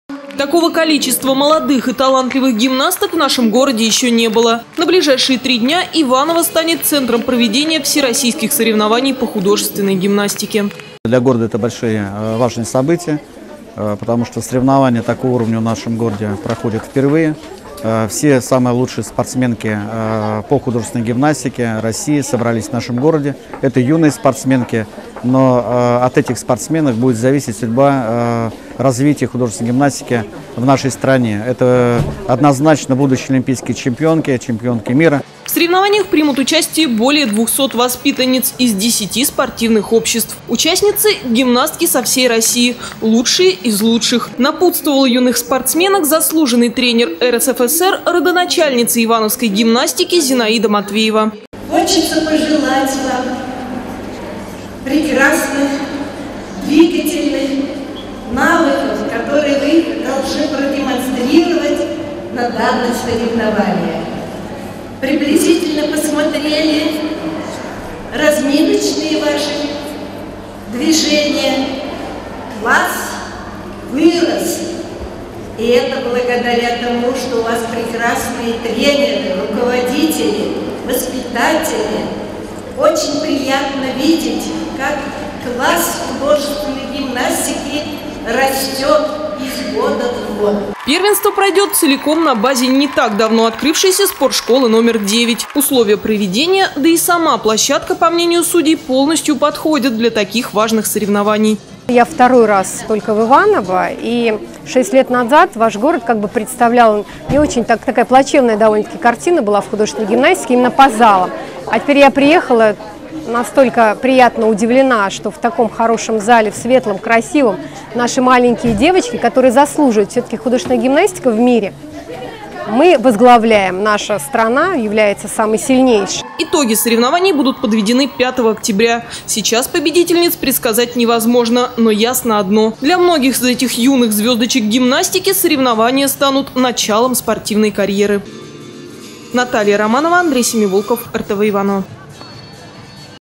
Всероссийские соревнования юных гимнасток
За три дня в соревнованиях примут участие более 200 воспитанниц из 10 спортивных обществ. Участницы -  гимнастки со всей России, лучшие из лучших.